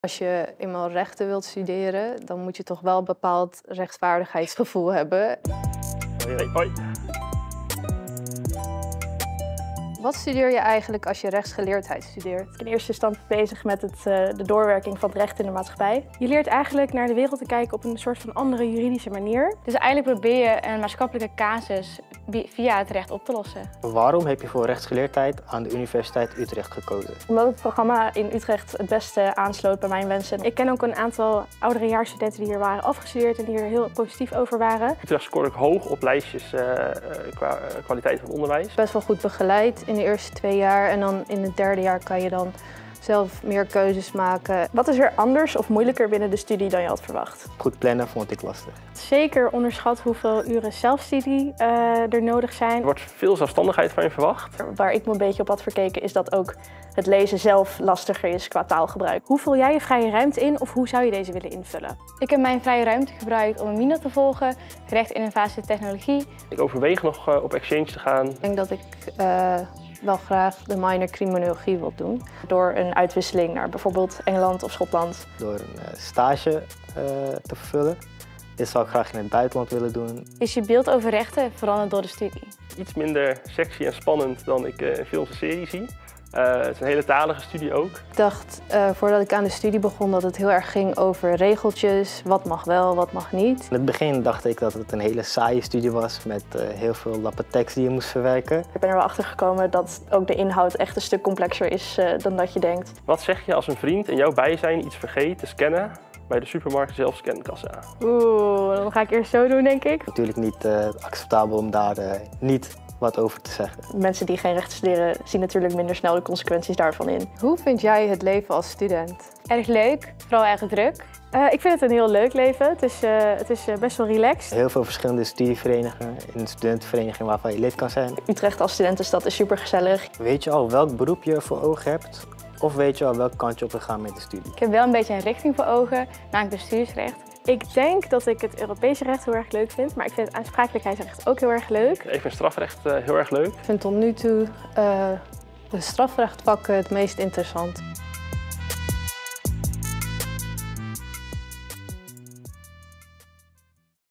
🔥Brandende vragen: Rechtsgeleerdheid
In deze video vertellen studenten van de opleiding Rechtsgeleerdheid over hun ervaringen met de opleiding. Wil je meer weten? Ga dan naar